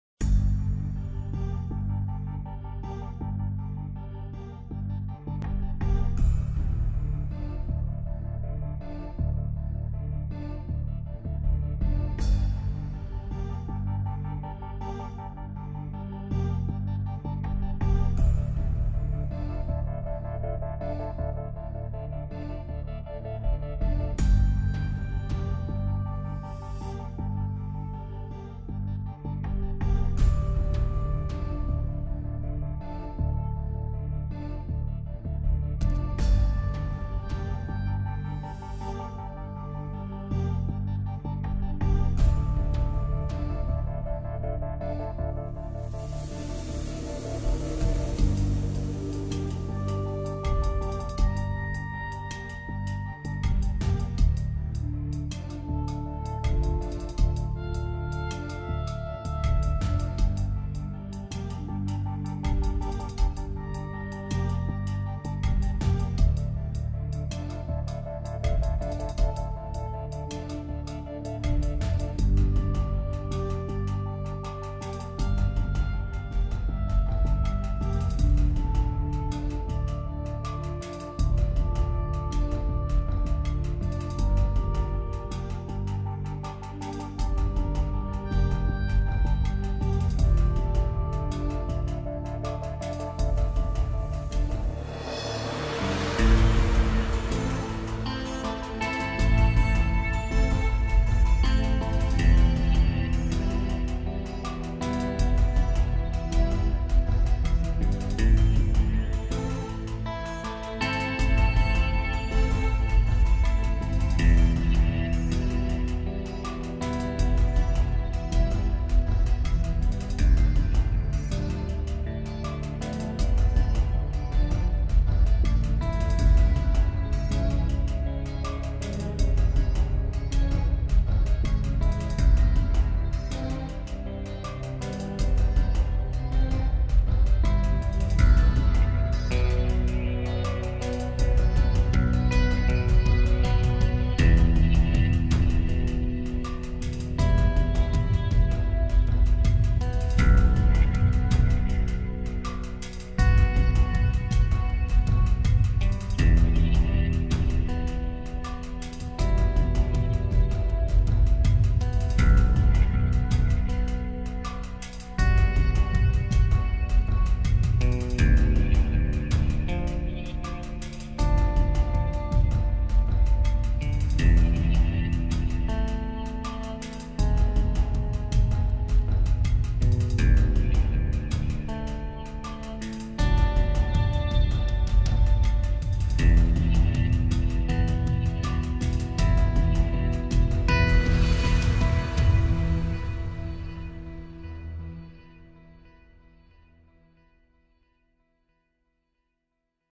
Speed Art Sharlto Copley |Agent Kruger Elysium
Speed painting agent Kruger from Elysium movie. He also acts in Sector 9. Extraordinary performance.